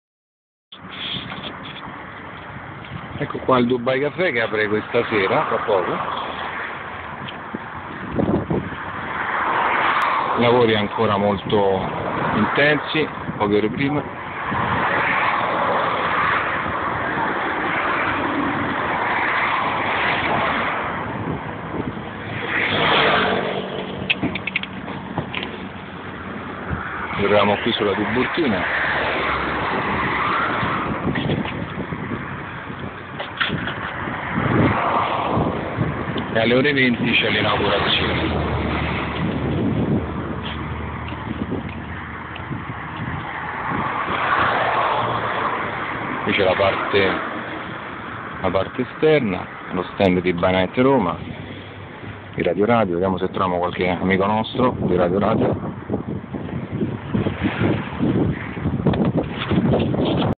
Bynight Roma : Dubai Cafè : video youtube : Tiburtina Setteville : Radio Radio
Preparativi al Dubai Cafè vicino Setteville di Guidonia a Roma, sulla via Tiburtina: nel video per youtube puoi vedere i preparativi dello stand del ByNight Roma di Radio radio, condotto da Emilio Giordani.